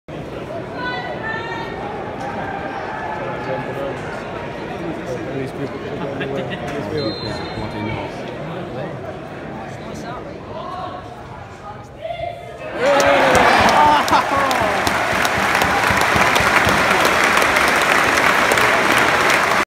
Kallum Higginbotham penalty Dunfermline vs Peterhead 17/07/18
Higgy chips the penalty down the middle to make it 2-0 in the Betfred Cup.